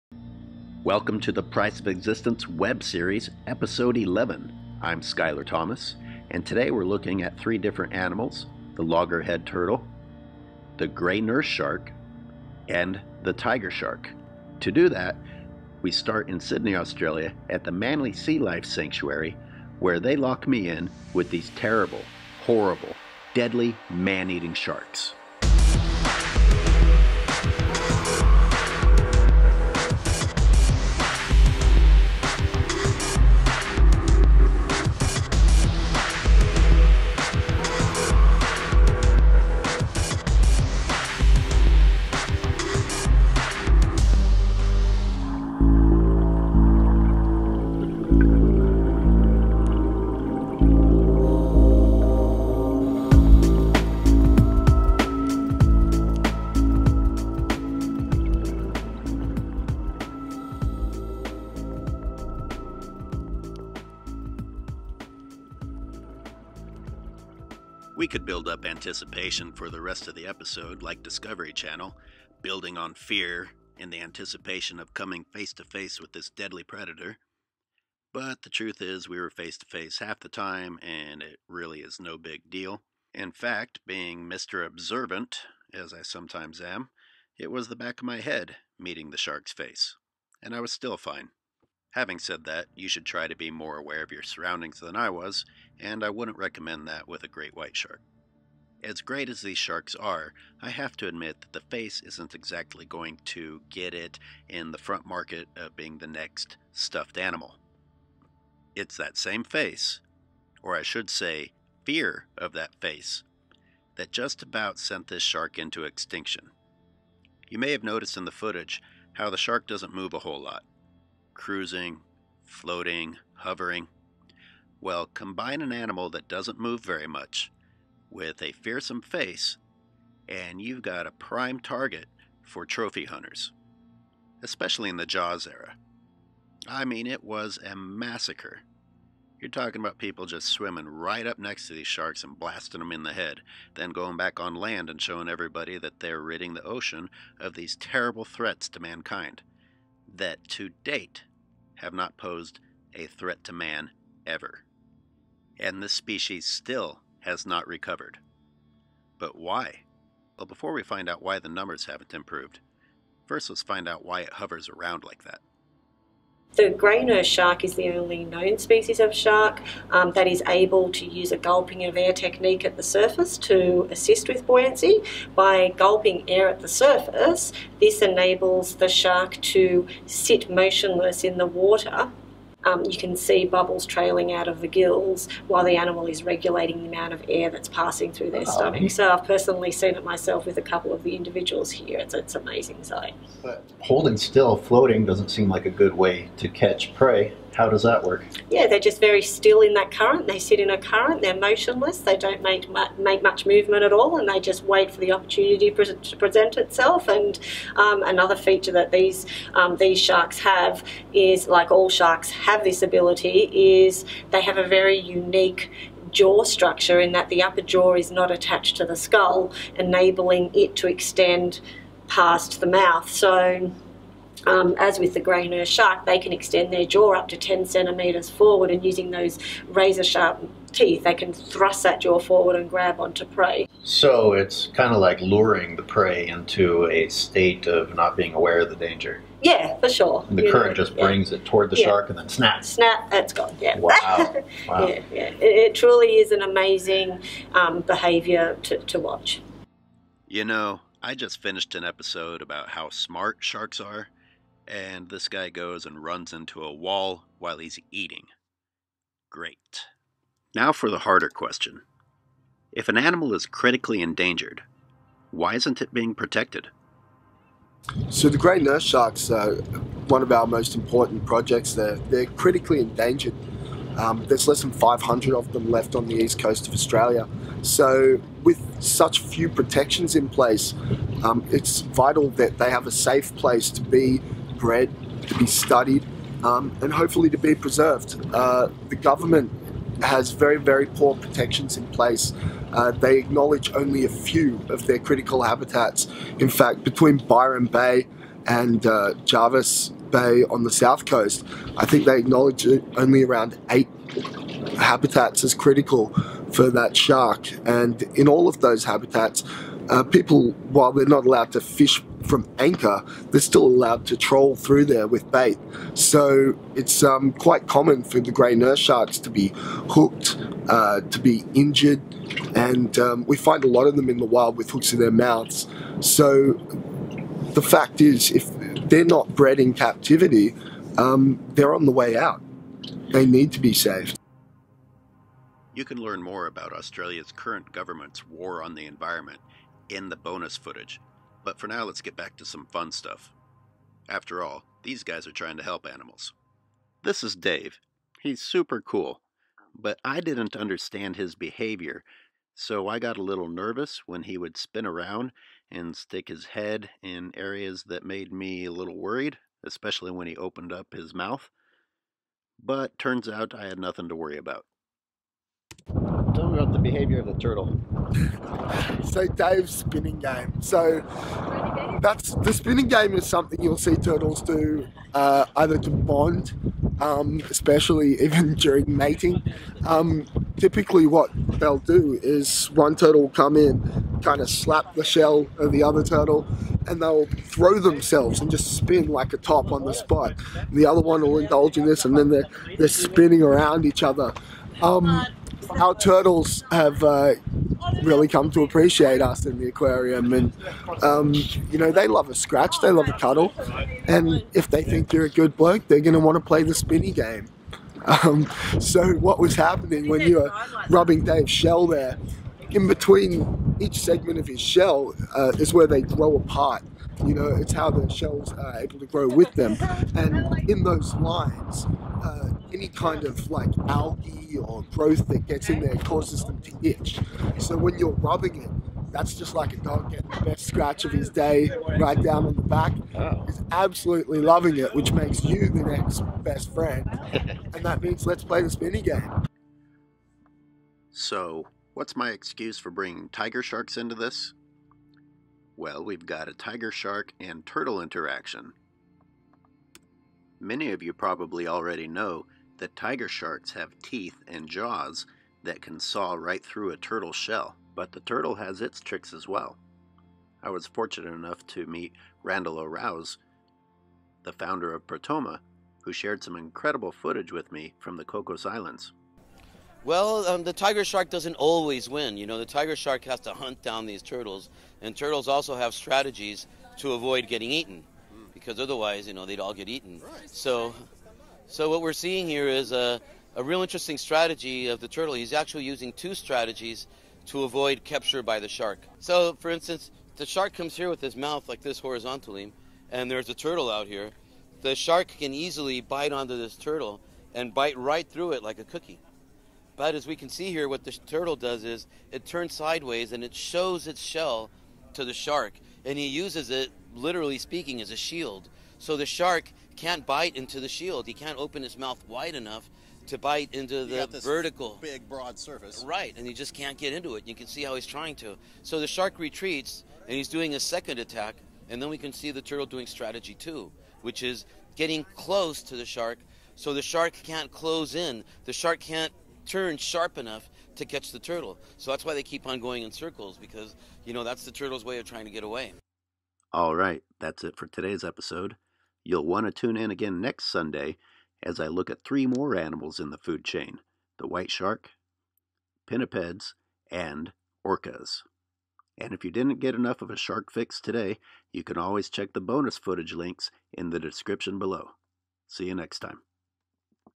Food Chain: Tiger Shark, Logger Head Turtle, and Sand Tiger Shark
An older episode from the Price of Existence series examining members of the food chain.  In this episode Skyler Thomas interviews local experts regarding the logger head turtle, tiger shark, and sand tiger shark.